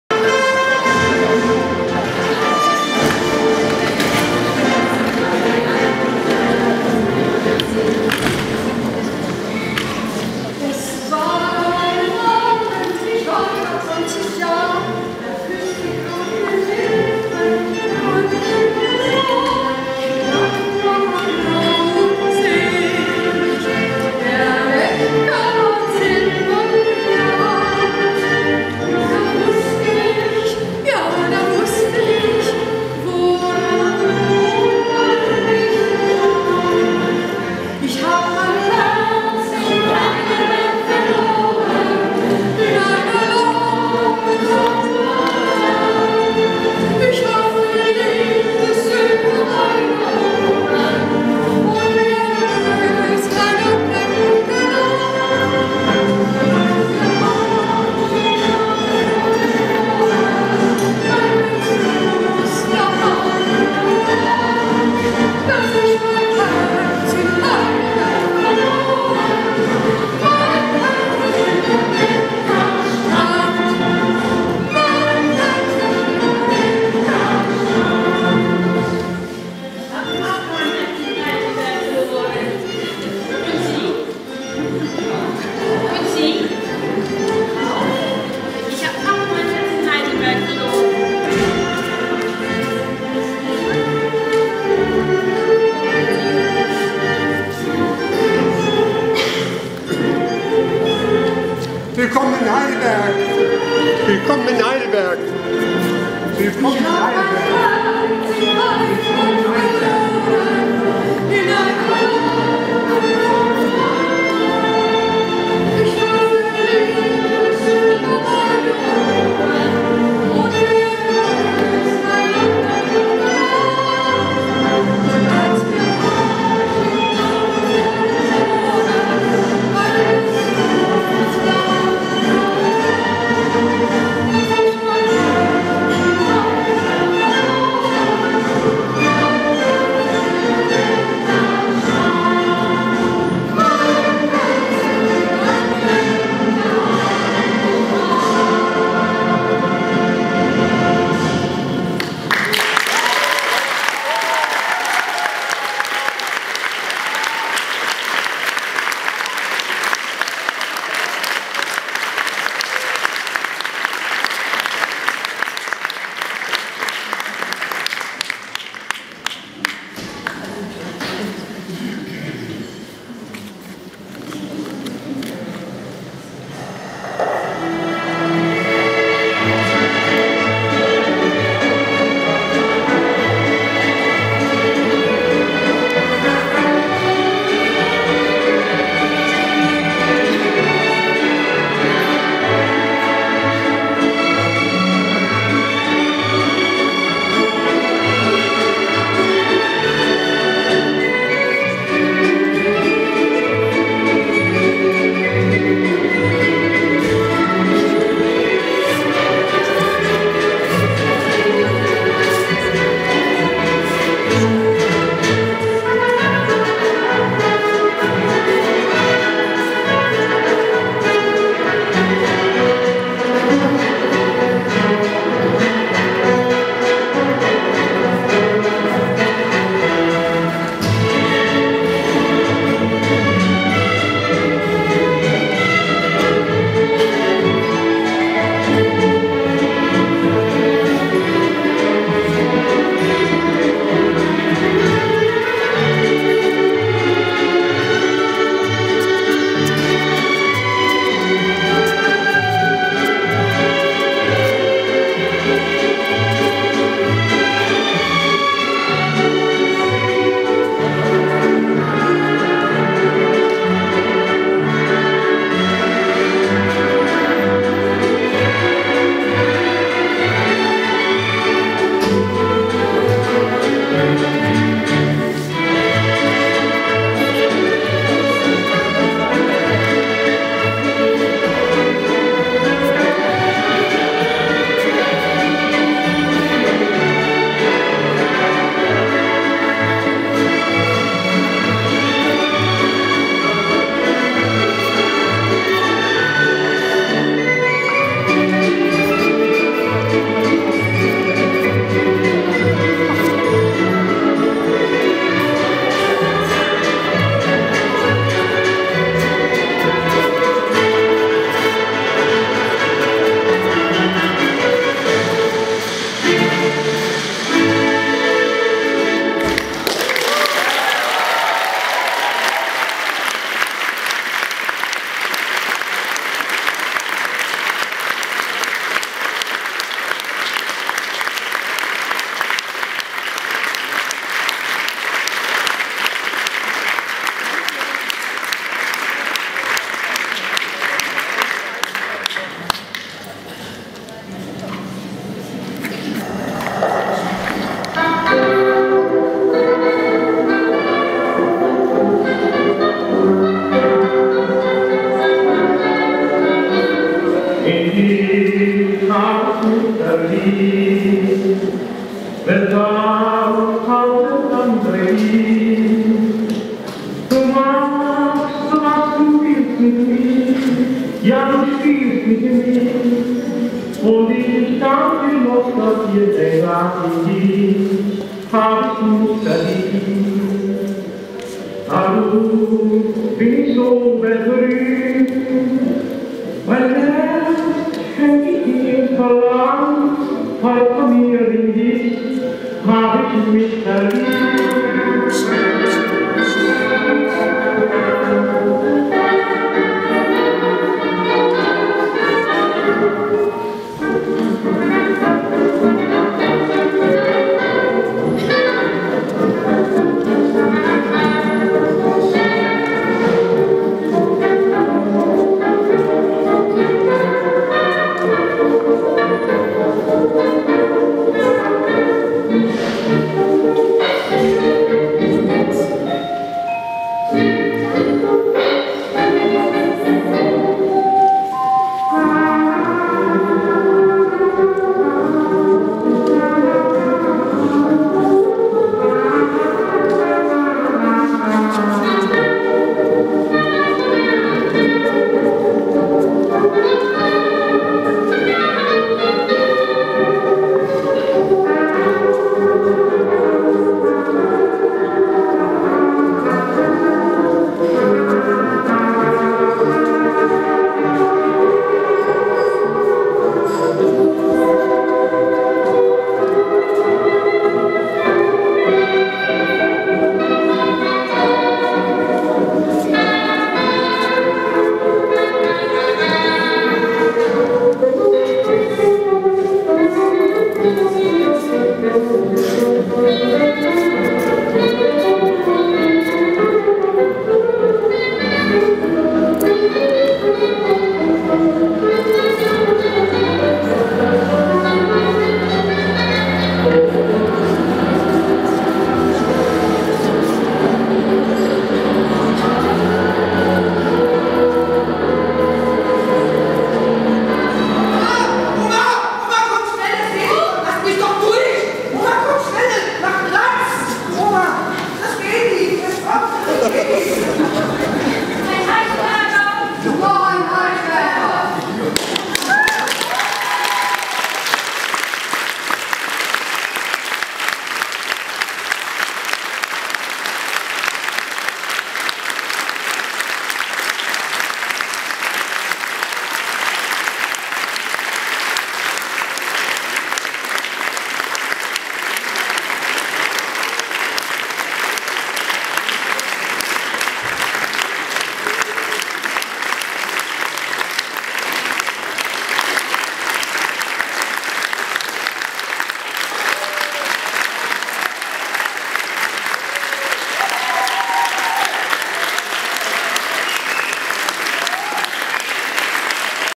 HD Moves - Heidelberger Herzen
Ein Tanzstück zur Tanzbiennale - HD Moves - Eröffnungsveranstaltung im Stadttheater Heidelberg - Dokumentation volle Länge -
Mit fantastischen Protagonisten im besten Alter (70+)